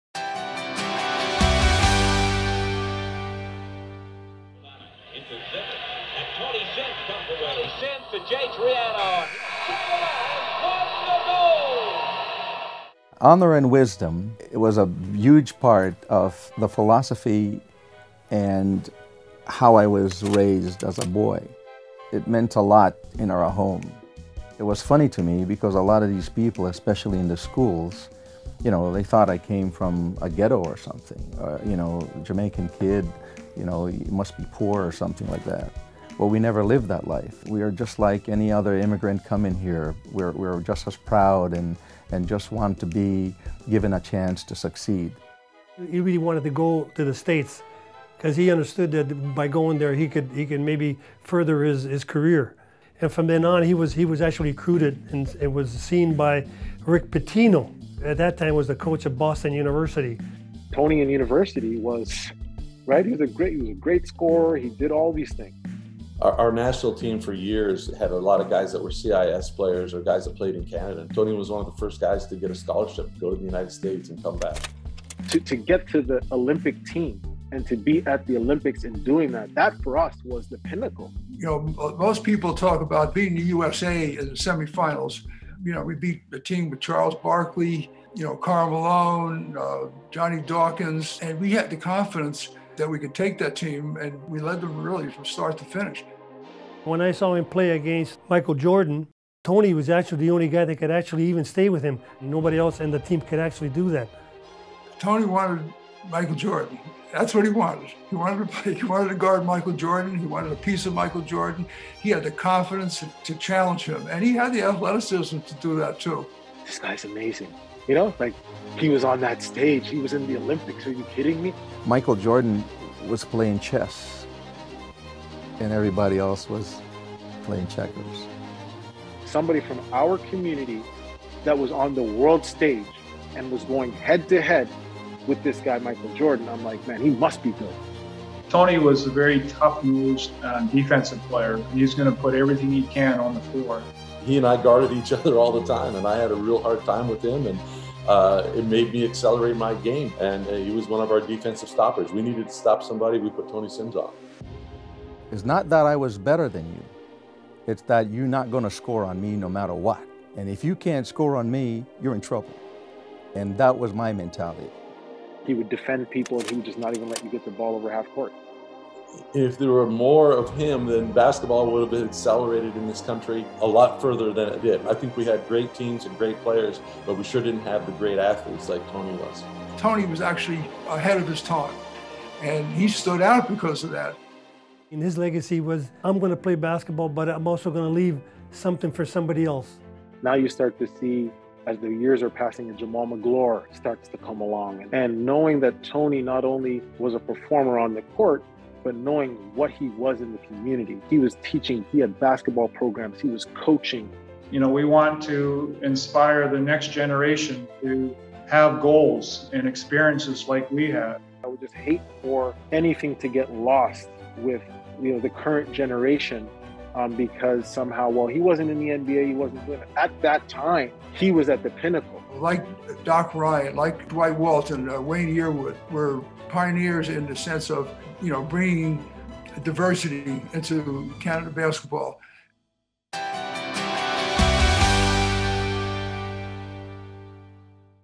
Honour & Wisdom: The Story Of Canadian Basketball Pioneer Tony Simms
Benji Agbeke's story and sitdown with Canadian basketball legend Tony Simms, who was one of the first U Sports players to get a scholarship to the U.S. and come back, and who had the confidence and athleticism to challenge Michael Jordan.